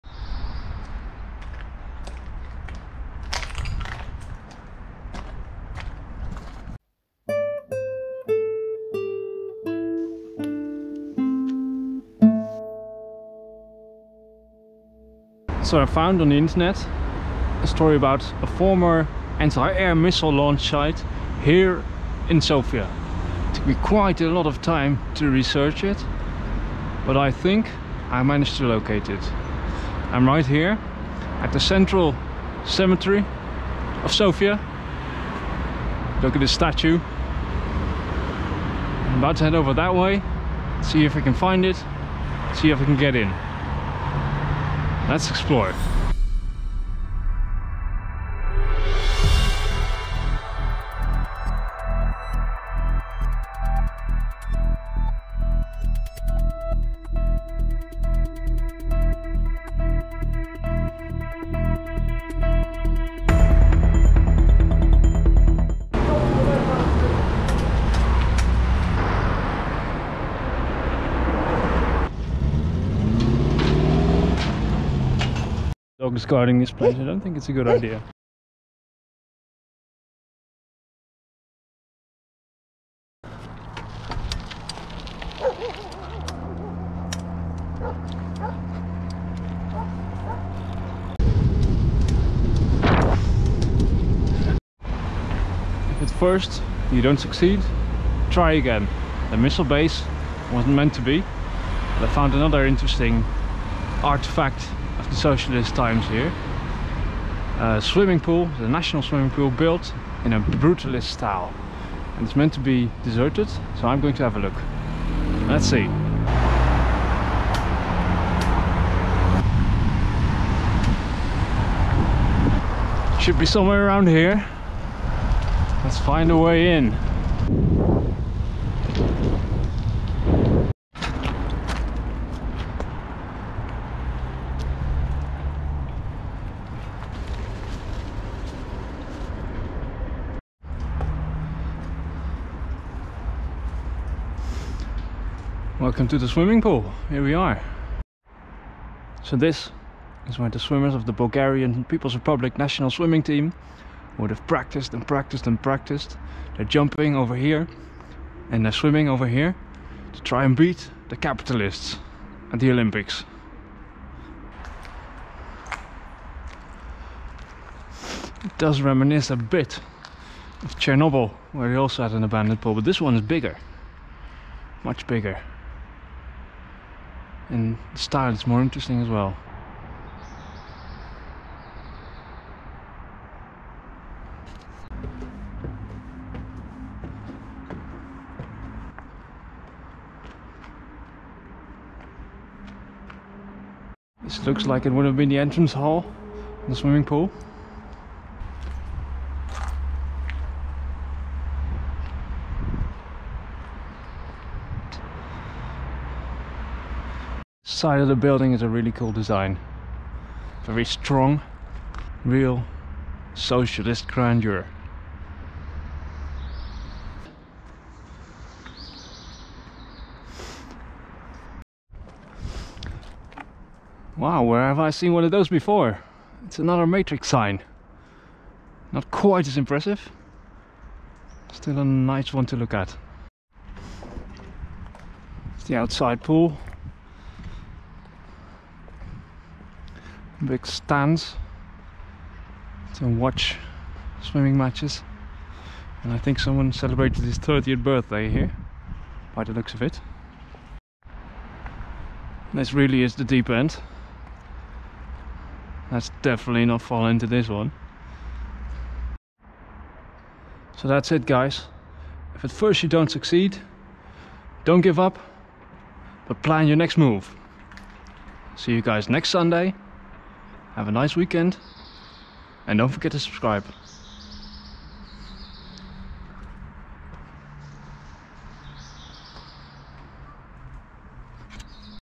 🇧🇬 Operation Igla
Much effort went into researching and planning an 9K38 Igla (NATO name SA-18) anti-air missile launch site in Sofia to visit. But plans can fall apart, as you will see in this video...

After the initial failure I instead visit the Pluven Kompleks Cherveno Zname (Red Flag swimming complex), also in Sofia.